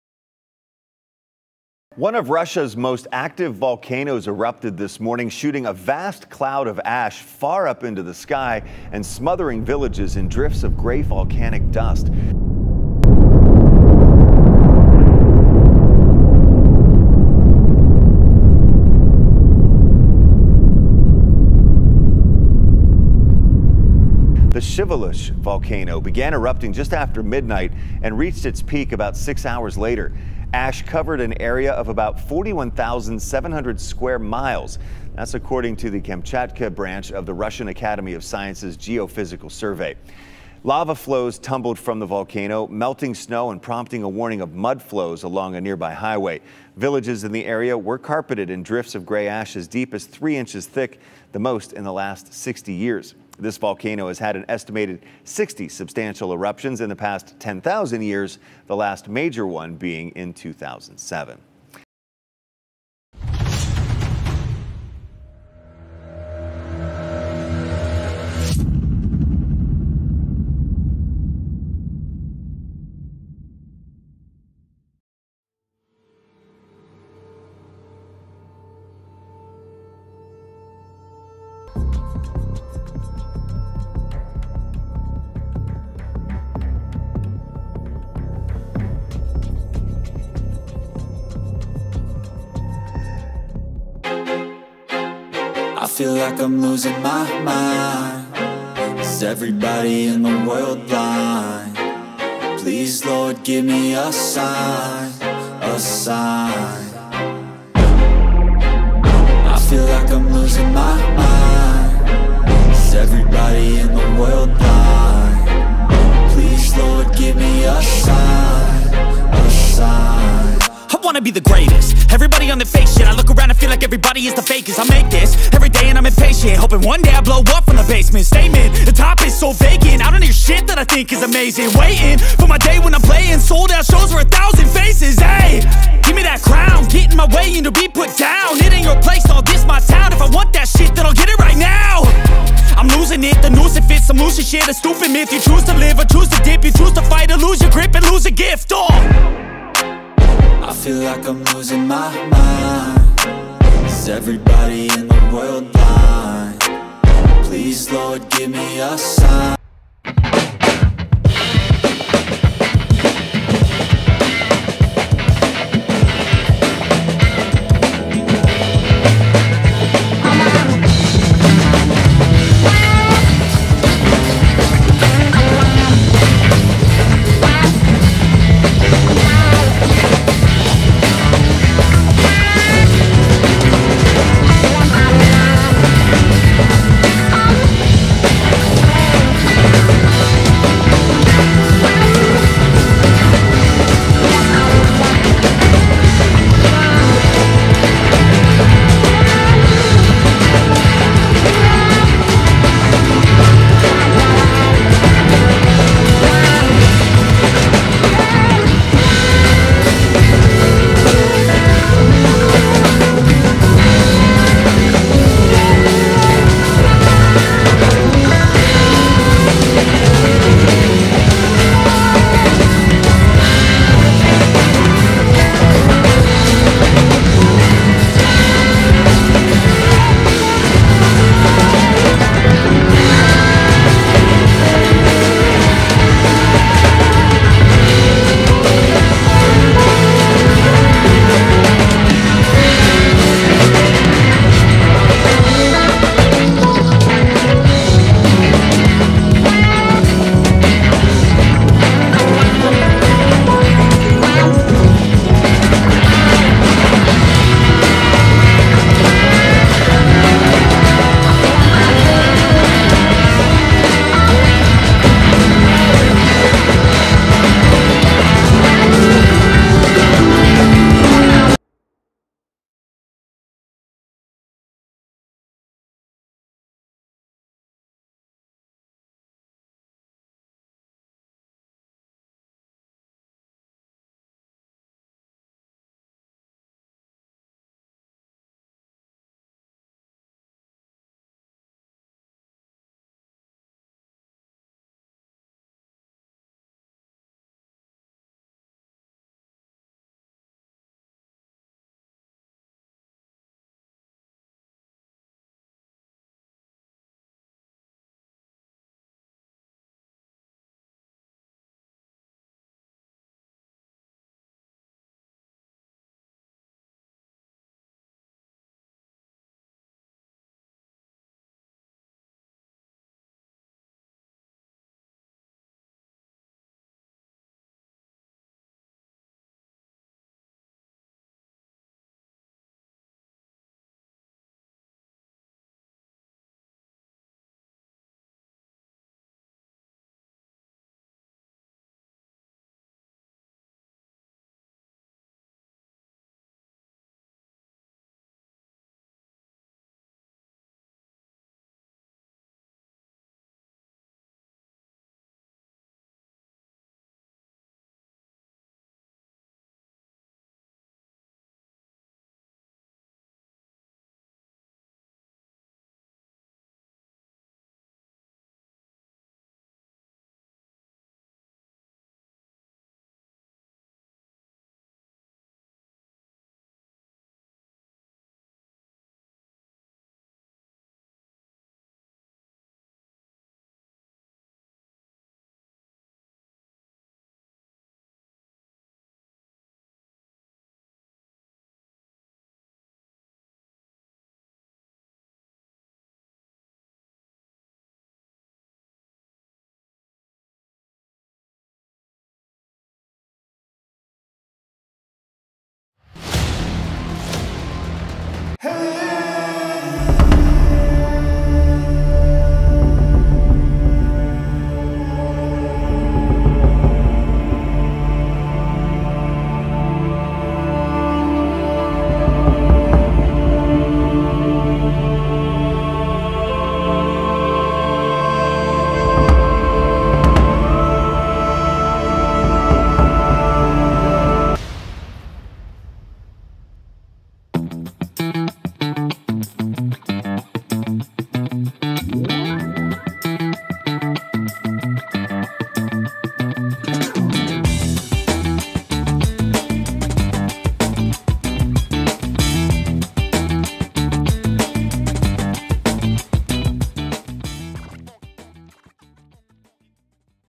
VIOLENT Shiveluch eruption UPDATE; Spectacular pyroclastic flows
Chasing massive pyroclastic flows of Shiveluch volcano on 11 April 2023, with the dog, literally just a few meters away!